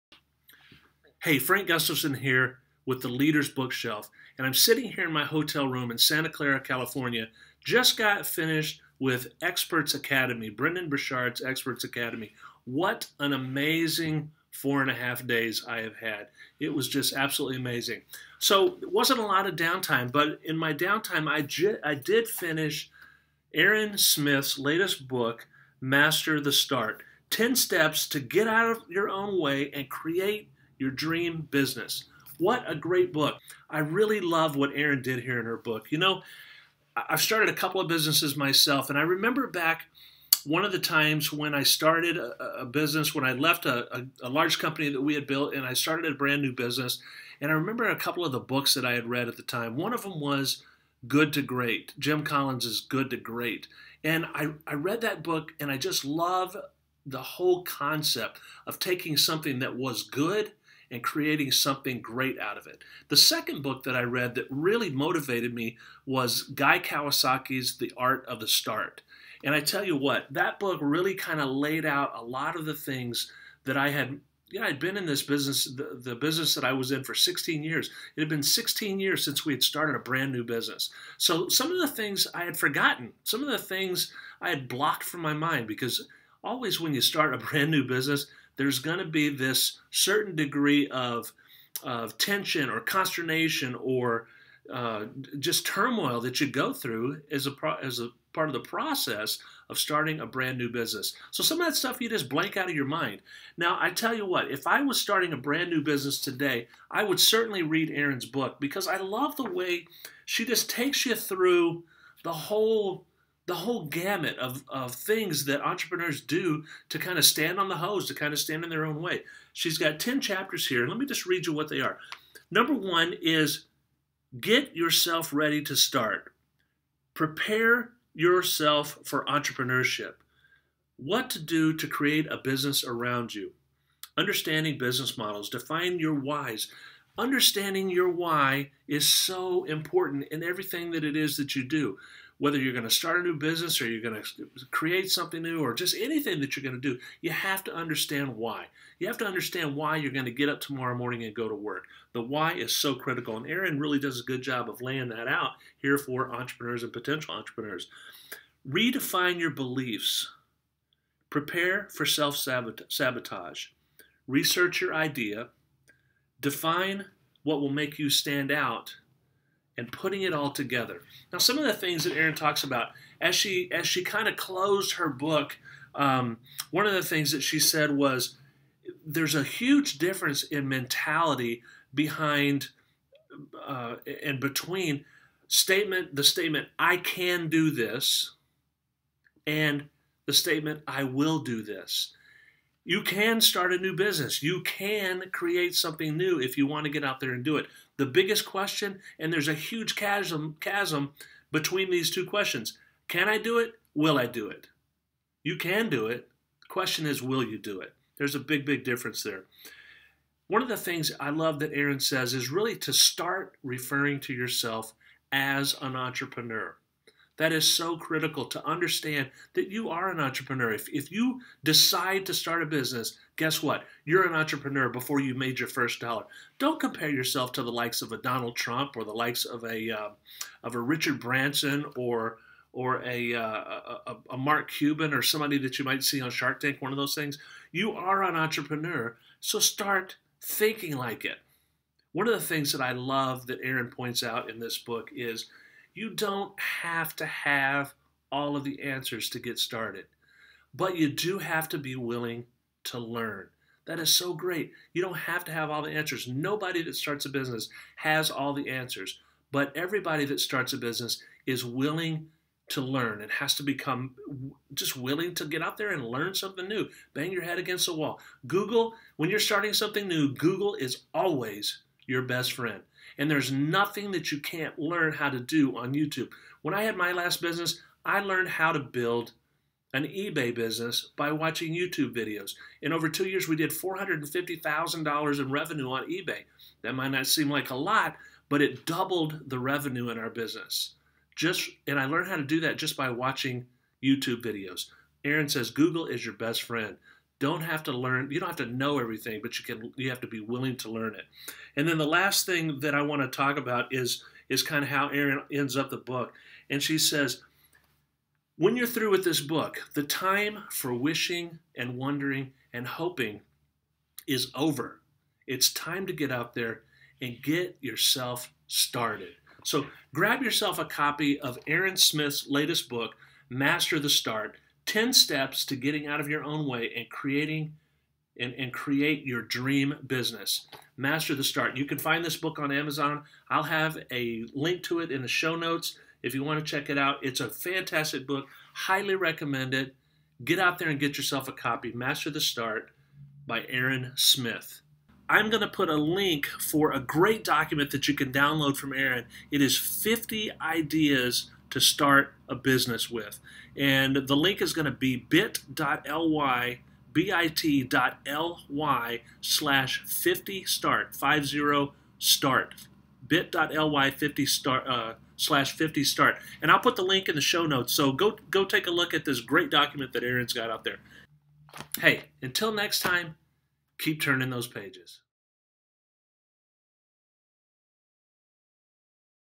TLB 025 Master the Start by Erin Smith - The Leaders Bookshelf
Erin Smith’s book Master the Start is a great read for anyone who is thinking of or who has recently started a business. Erin offers up 10 ways that entrepreneurs ca get out of their own way and create their dream business.